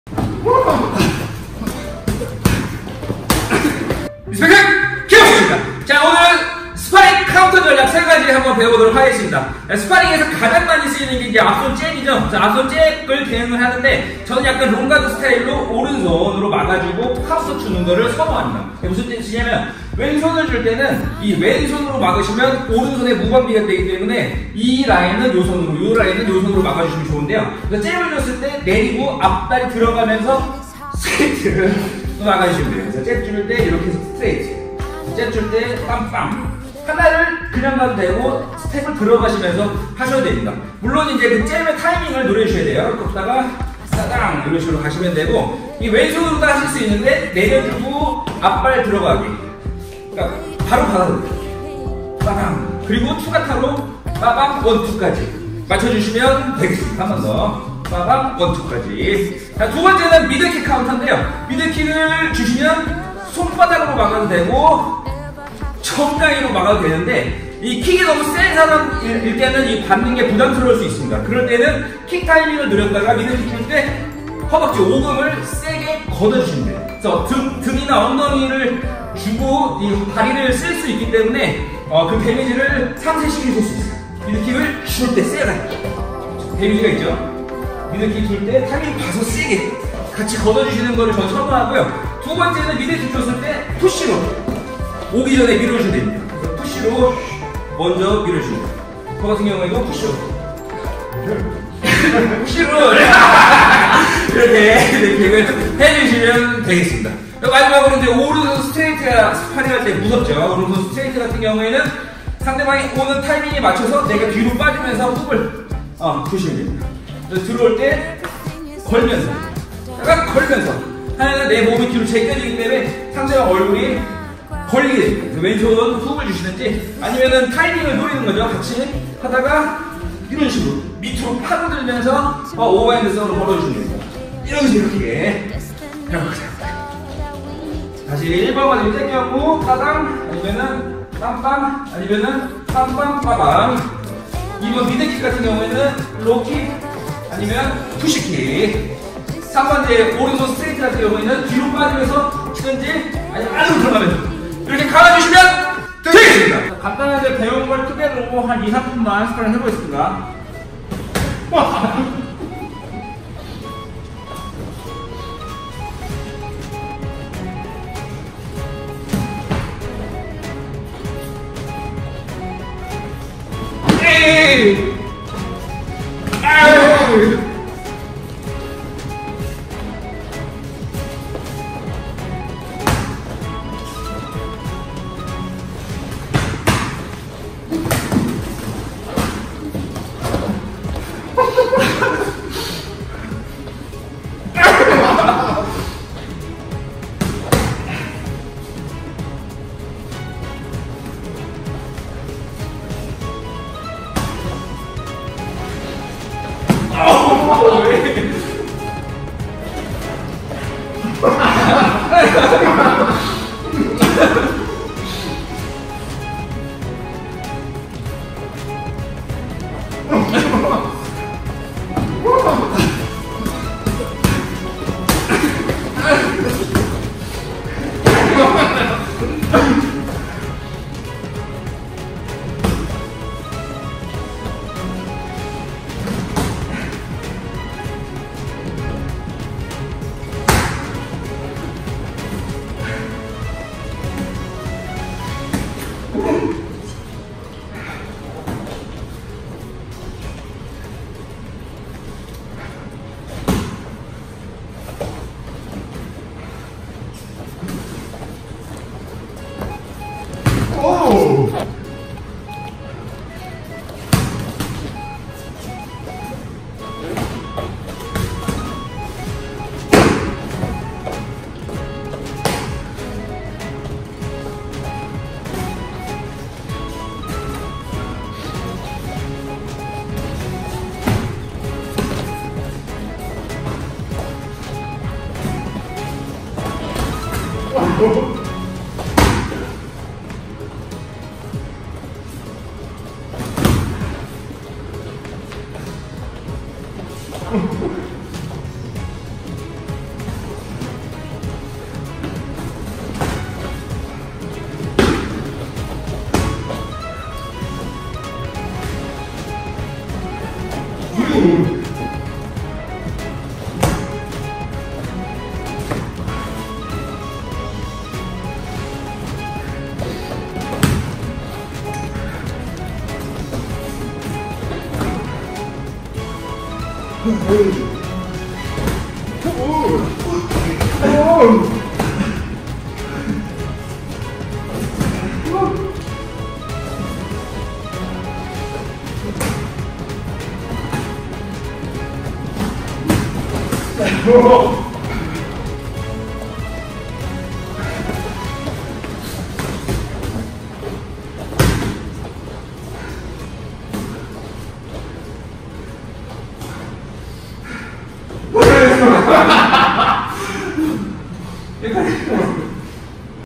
분위기를 반전시키는 스파링 카운터 전략 3가지
1. 잽카운터 - 오른손으로 내려주고 투/잽/잽잽투
2. 미들킥 카운터 - 킥타이밍에 디딤발 걷어주기 / 푸시킥
3. 크로스 카운터 - 뒤로가며 L훅 / 앞으로가며 R오버핸드훅